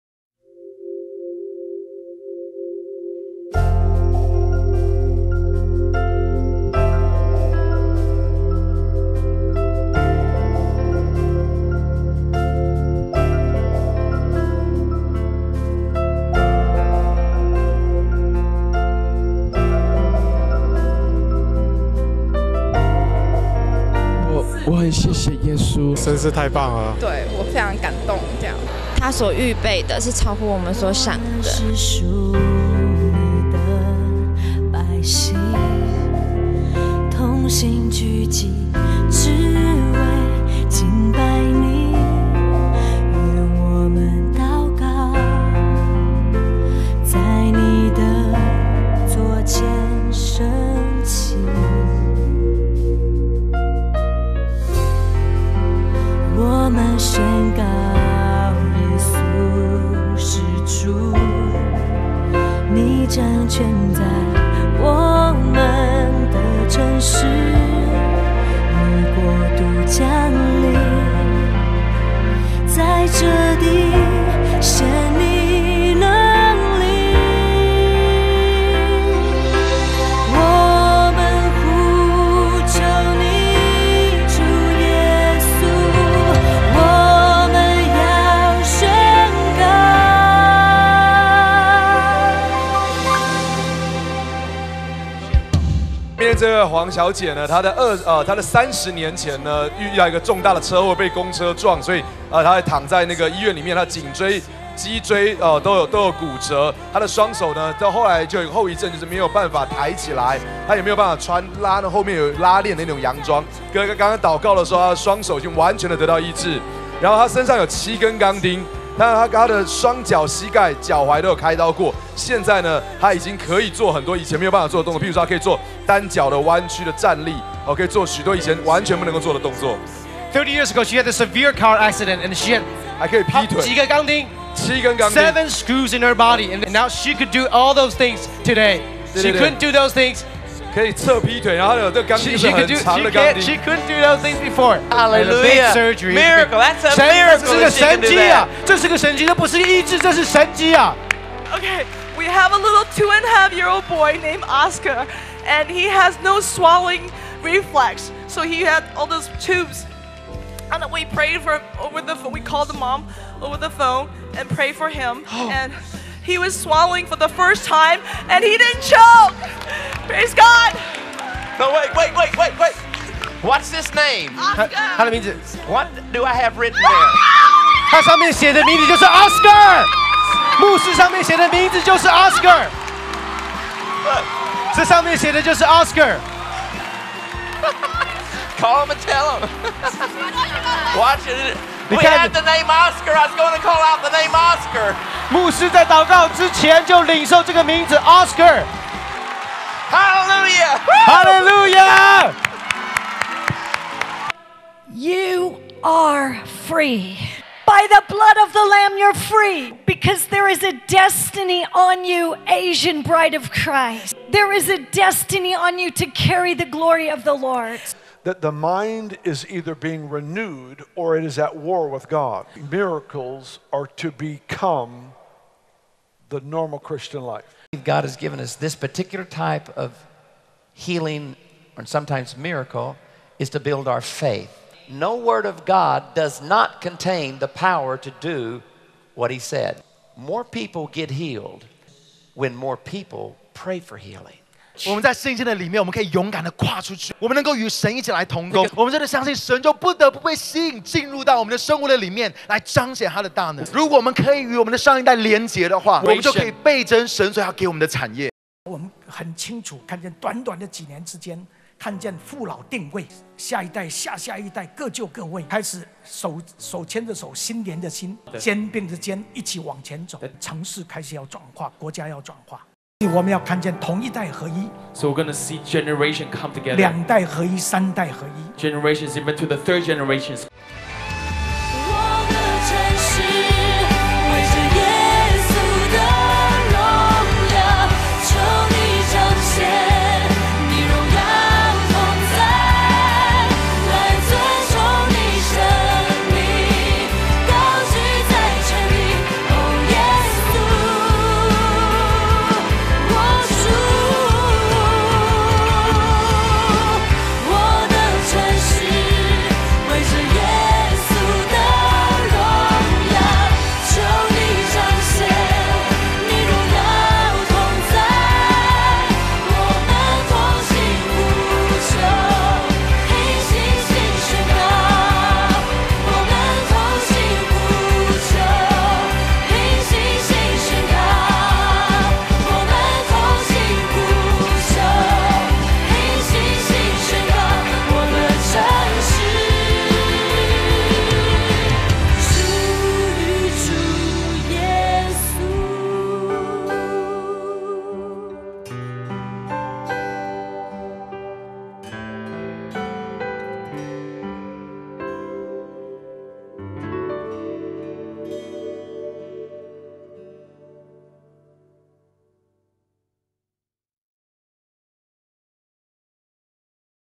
2012 | [ 天國文化特會 ] - 回顧
2012天國文化特會&醫治佈道會
時間:2012年 1/31(二) ~ 2 / 3(五)
地點:高雄巨蛋-高雄市左營區博愛路2段757號
超強講員:周神助牧師(Nathaniel Chow)、柯蘭迪(Randy Clark)比爾‧強生(Bill Johnson)、海蒂‧貝克（Heidi Baker）、周巽光牧師(Ewen Chow)、 Sidney Mohede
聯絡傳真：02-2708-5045
2012天國文化信息訂購